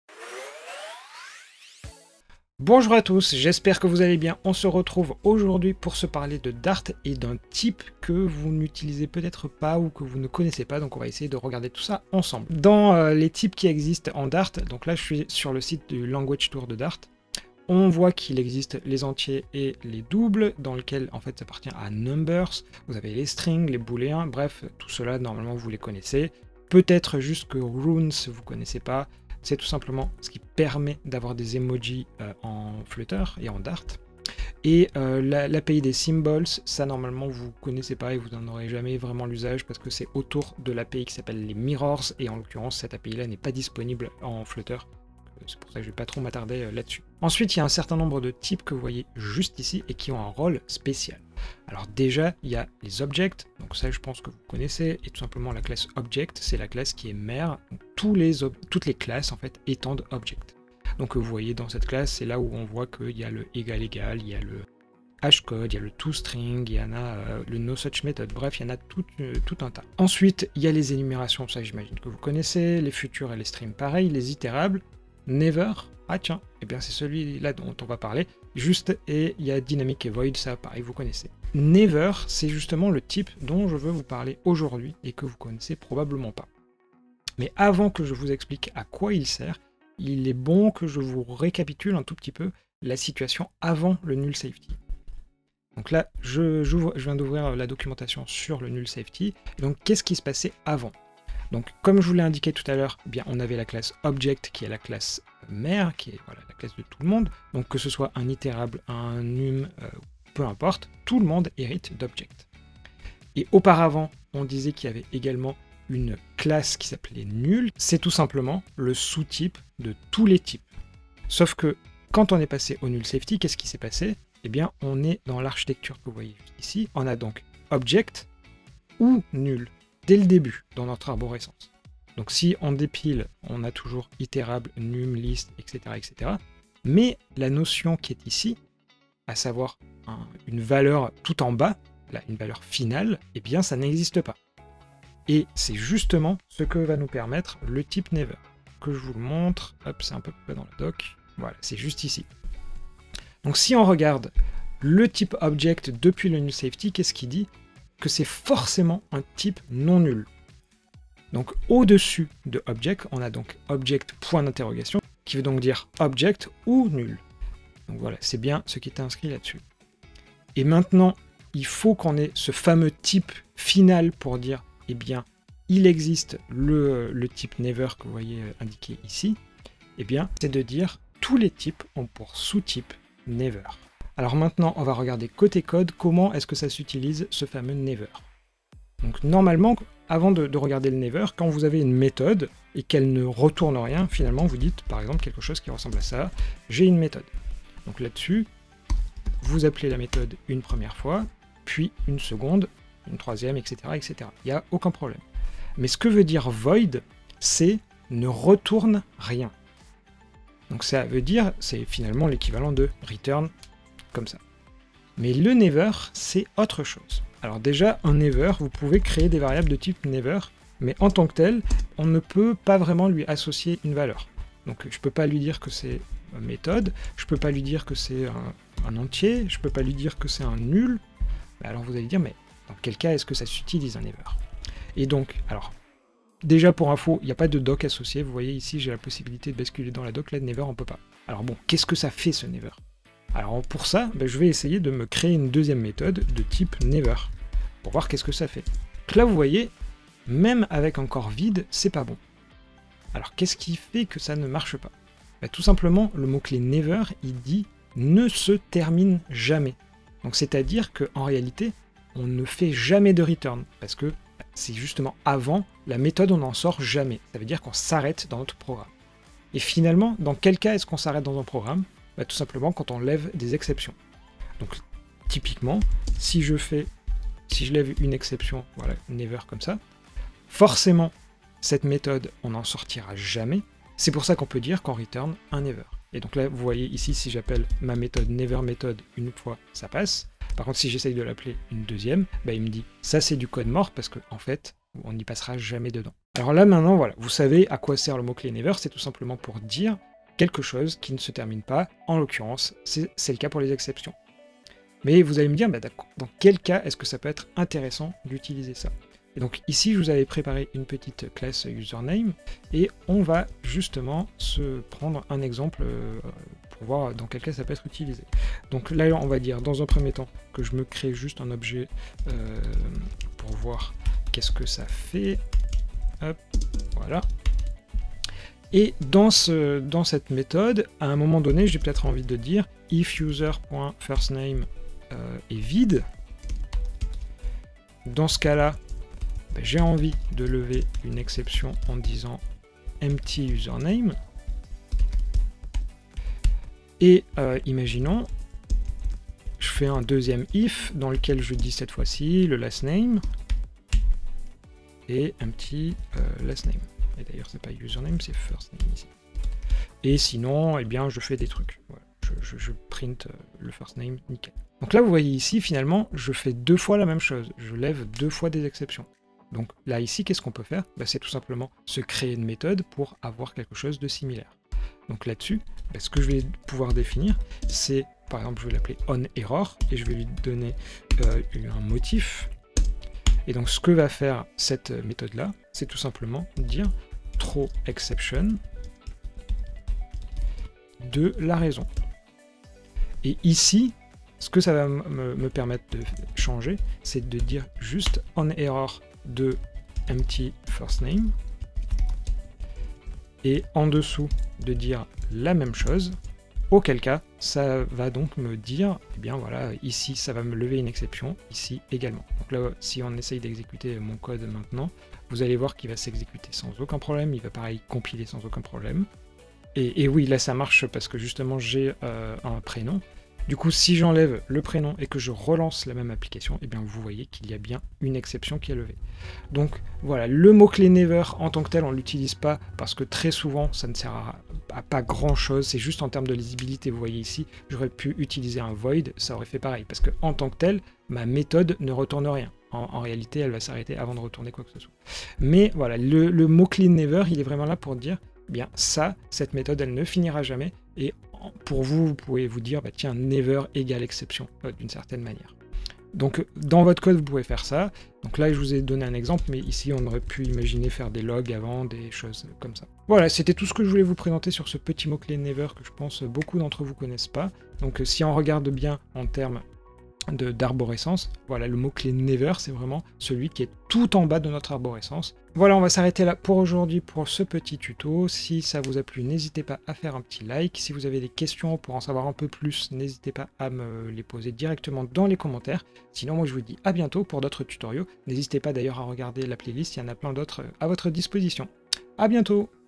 Qu’est-ce que le type Never en Dart ? 👨‍💻 Tutoriel Dart/Flutter
En Dart, il existe de nombreux types dont int, bool, String… Il existe également des types avec des rôles spéciaux dont dynamic, enum, Object…. Mais dans cette liste, on observe qu’il est question d’un Never. Arrivé légèrement avant le null safety (Dart 2.9 vs Dart 2.12), nous allons voir ce qu’il permet comment l’utiliser dans vos applications;

N’hésitez pas à liker la vidéo pour que d’autres personnes puissent la découvrir.

🗓 Au programme de ce tutorial :

00:00 Introduction
00:12 Les différents types en Dart (number, String, Runes…)
00:48 Les types “spéciaux” en Dart
01:23 Le type Never
01:36 Les types avant le null safety
02:30 Les types avec le null safety
03:04 Démo de code du Never
04:57 Un exemple d’usage du Never
07:12 Conclusion sur Never
07:45 A bientôt